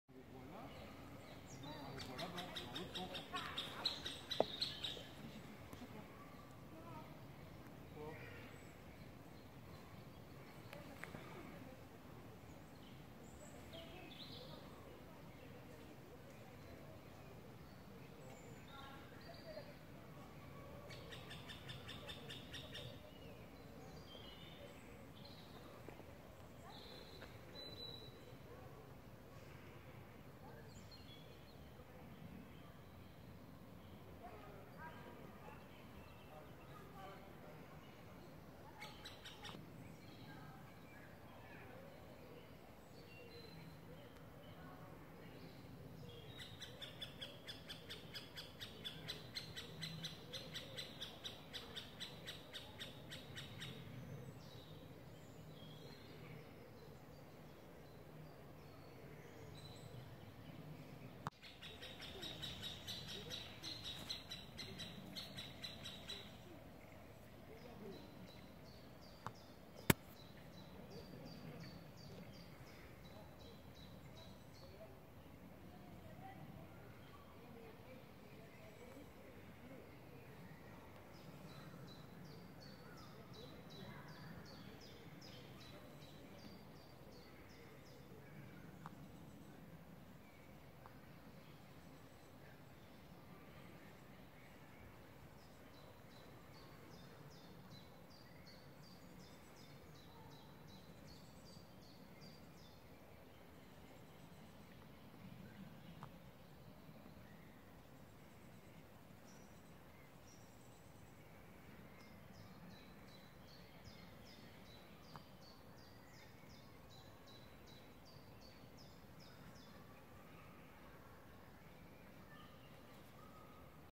Pic mar (dendrocopos medius)
Pic mar. poussin tombé du nid essayant de remonter le long d'un arbre proche. Parc de la Poudrerie (93) le 22 Mai 2019